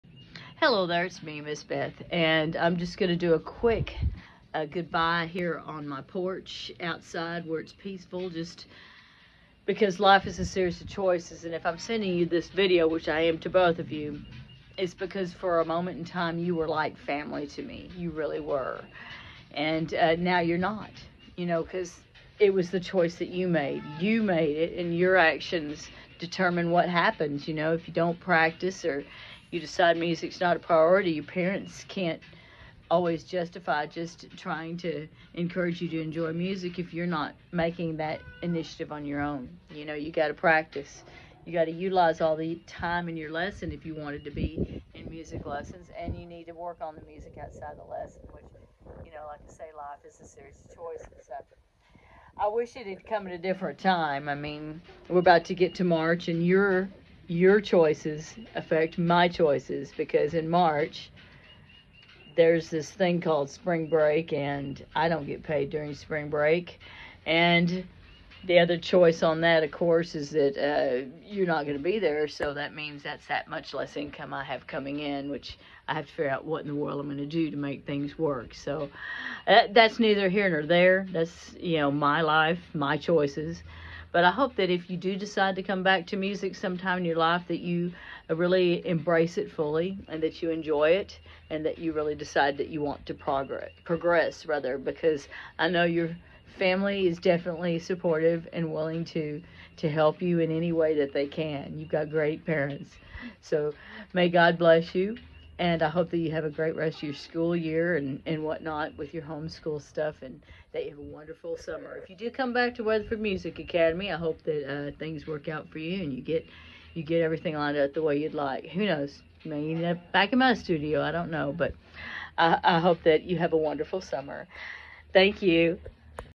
Goodbye video
Music instruction has its season for some students.  These videos are hard to do for me- I cannot comprehend a life without music in it